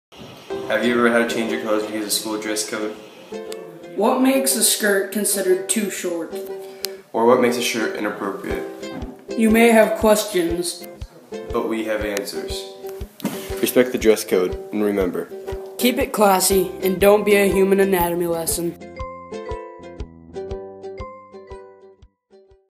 "Nothing But the Truth" project
Dawson and Tye demonstrate a correct way to interpret a school rule.  Inspired by the novel "Nothing But the Truth"by Avi  in Reading 8.